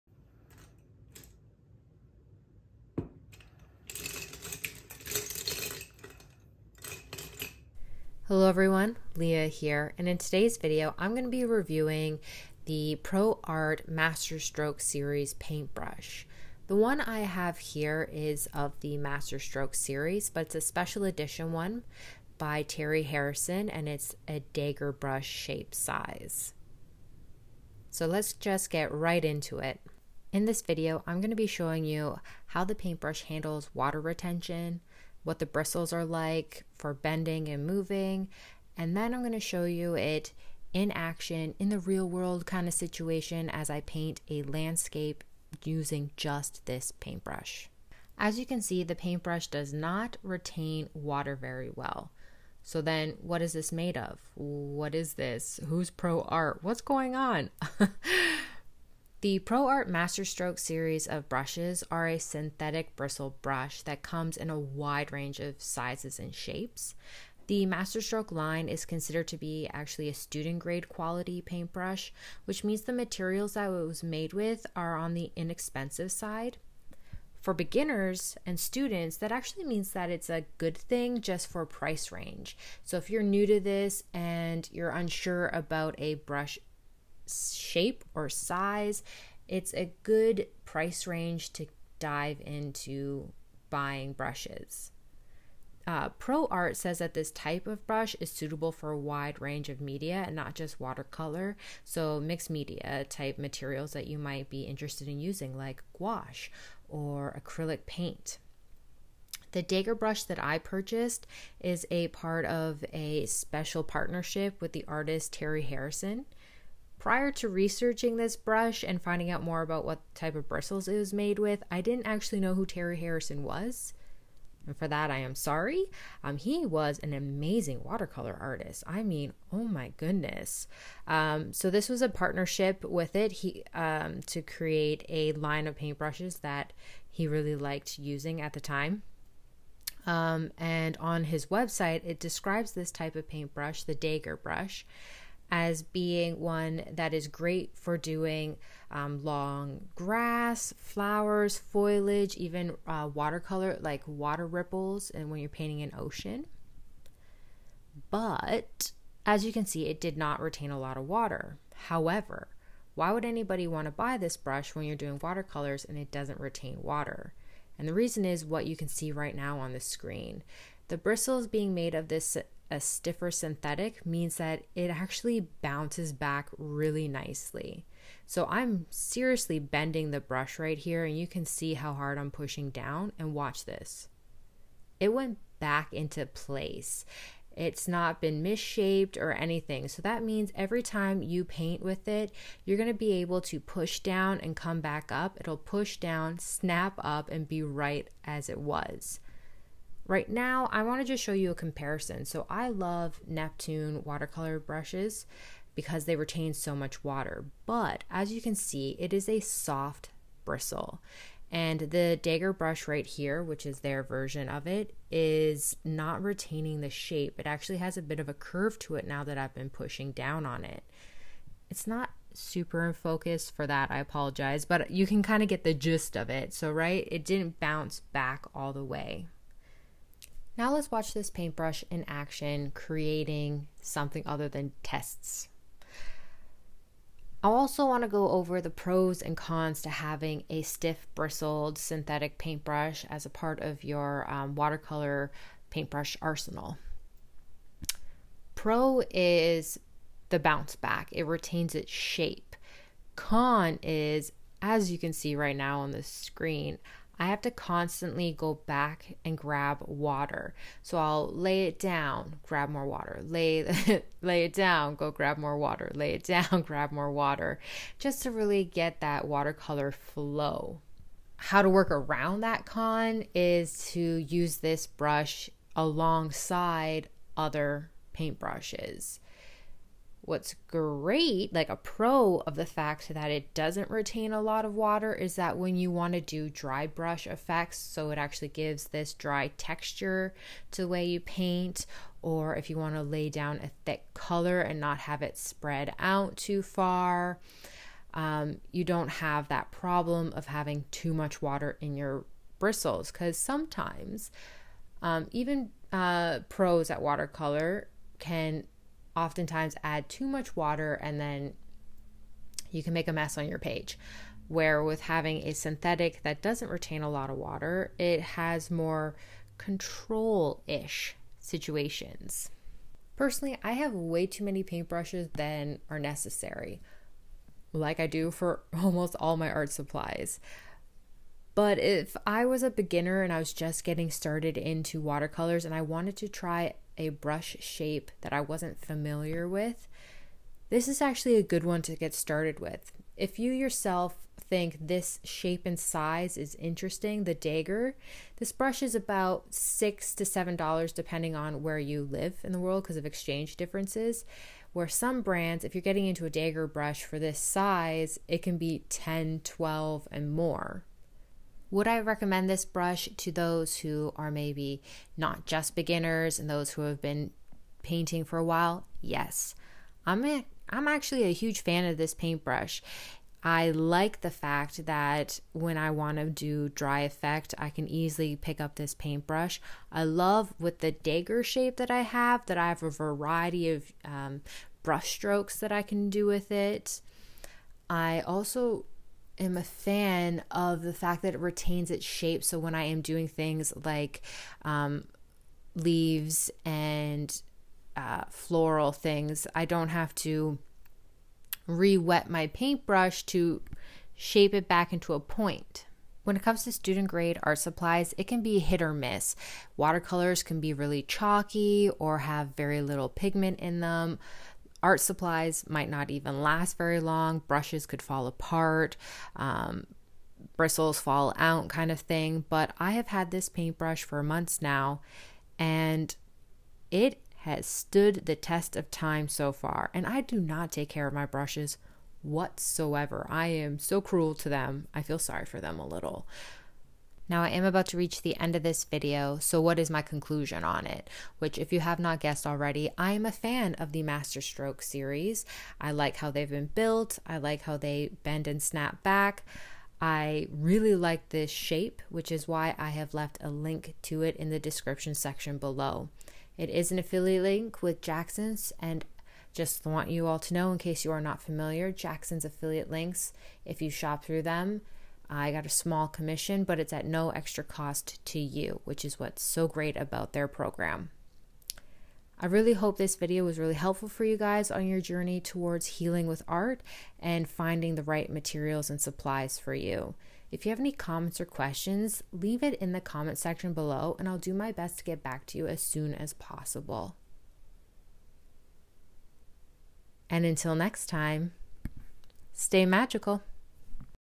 Pro Arte Masterstroke Watercolour Paint Brush Review - Perfect Watercolour Brush For Beginners?
Is Pro Arte Masterstroke paint brush perfect for beginners? The Masterstroke is a synthetic watercolour paint brush. How does this synthetic brush compare to Princeton’s Neptune watercolour brush? In this video we examine the Dagger Brush that Terry Harrison created specially for the Masterstroke series of paint brushes by Pro Arte. This paint brush was created for beginner watercolour artists. With tests, comparisons and real life painting demo, you will be able to see if this is the right brush for you.

For the Free Healing With Art resources and activities

Where you can also find me @leahevergreenart = Instagram & TikTok

Product being Reviewed:
Pro Arte Masterstroke - Terry Harrison Special - Dagger Brush

Other Products Used In This Video:
Etchr Portrait Sketchbook B5 Cold Press

Princeton Neptune Dagger Brush (shown as a comparison brush)

Spring Watercolour Palette:

Lilac

Mineral Violet

Shadow Violet

Aquarius Green

Sap Green Light

Deep Green Gold